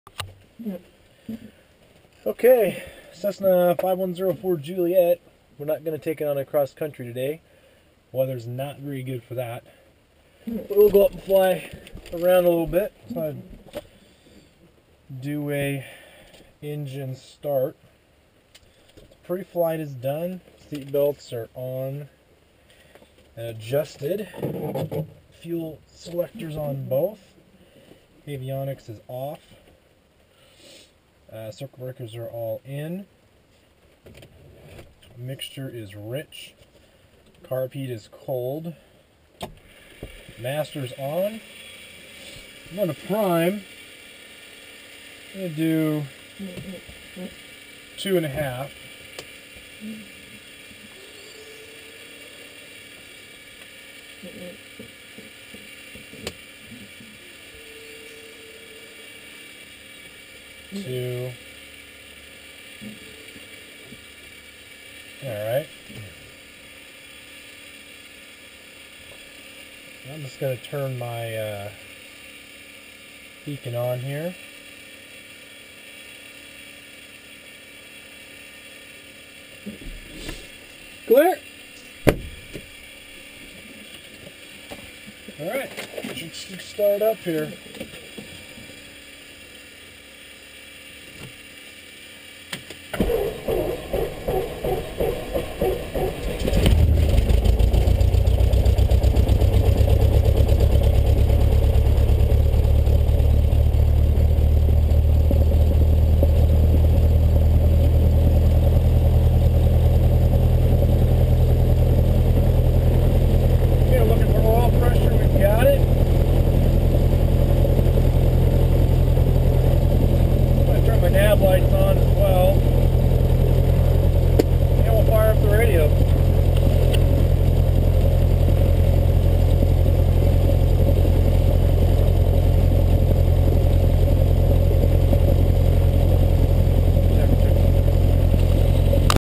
Start up for a Cessna 172
Just a quick video of the start up process for a Cessna 172.. Checklists, checklists, checklists